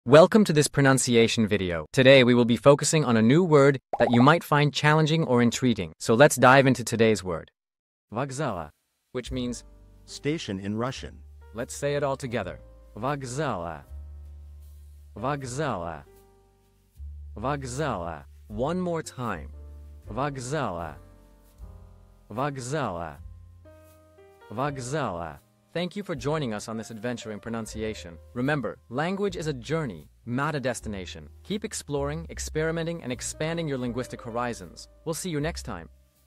How to pronounce Vokzala | Вокзала (Station in Russian)
Master the Pronunciation of 'Vokzala | Вокзала' - which means : Station in Russian 📝 with @PronunciationRussian 🎓🗣️ - [Your Guide to Russian]

Learn to pronounce 'Vokzala' perfectly in Russian with our easy and engaging tutorial by @PronunciationRussian! 📖📽️ In this comprehensive video, we provide you with native speaker examples 🗣️👂 and phonetic breakdowns 🎵 to ensure you get the pronunciation just right.